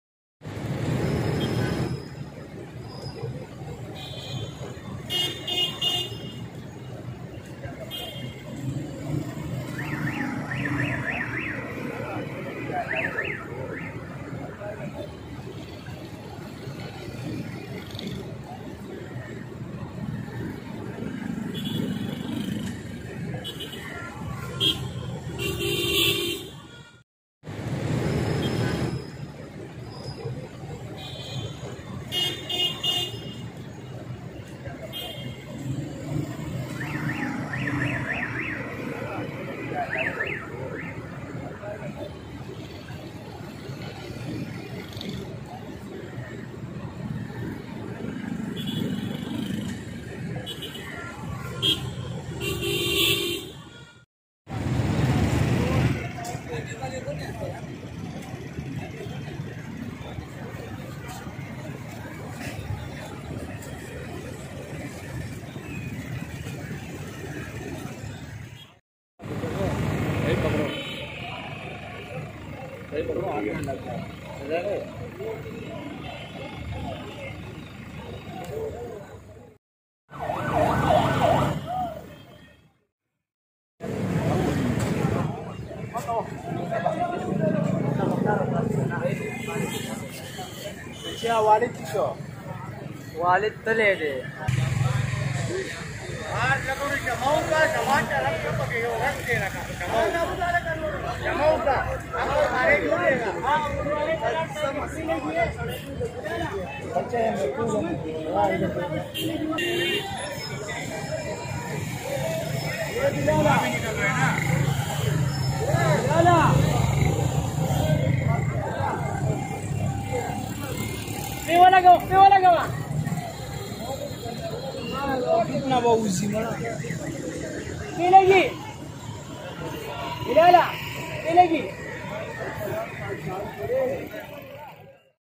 KPT underpass situation on 28-08-2020 | Underpass close
Information some children drowned in KPT underpass

Metropolitan
KMC